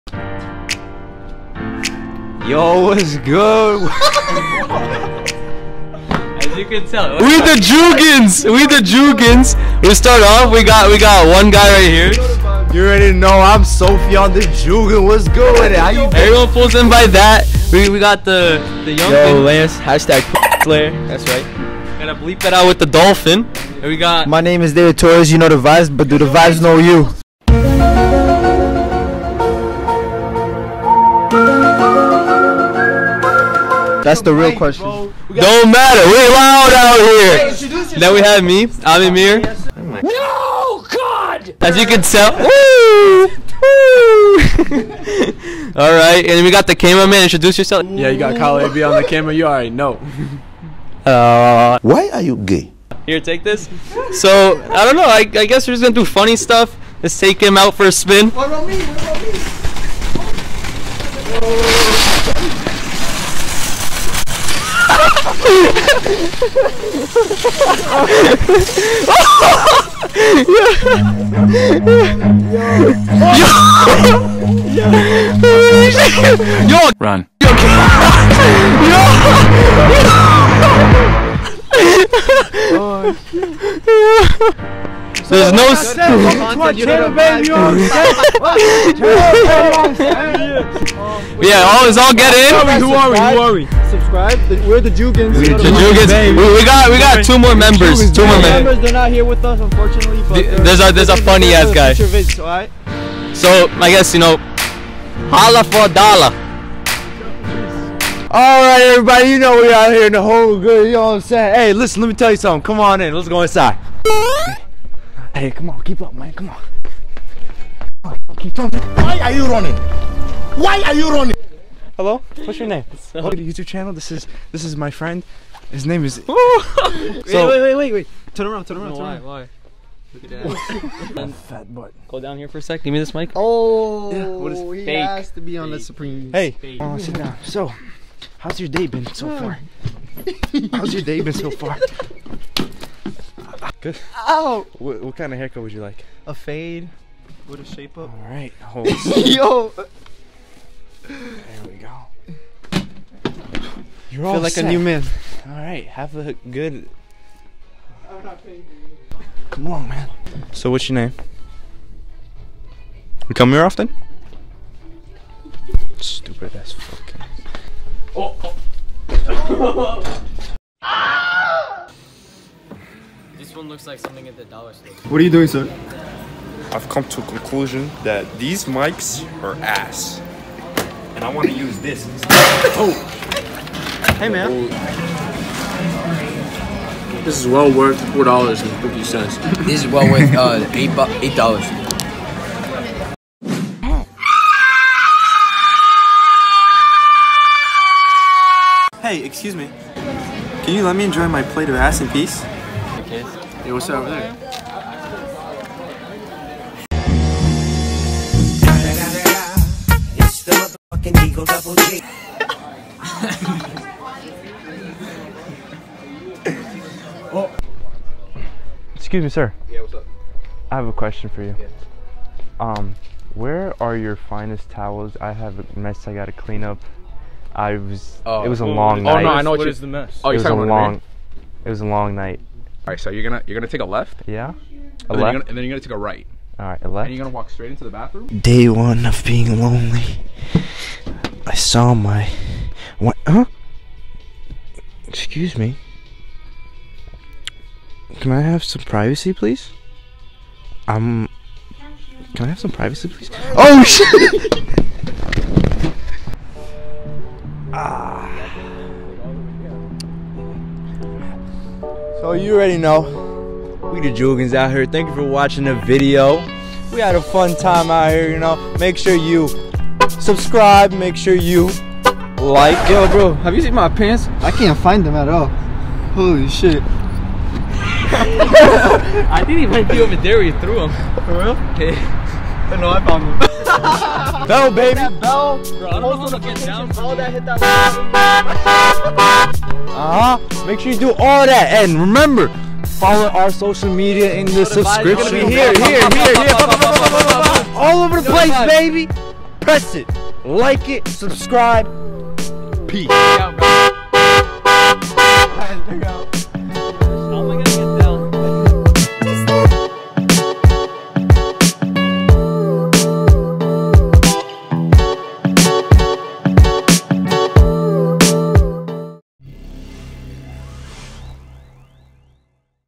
FIRST VIDEO GONE WRONG!!! SHOPPING CART CRASH & MORE!!!
Join us on our first of many funny adventures!
Members
Kyle AB - @kmart2118
Soufiane - @sofine_sofa
David - @d_torres.203
Emir - @emir.hrustemovic
Lance AB - @lance.AB_10

Check out the Joogans @thejoogans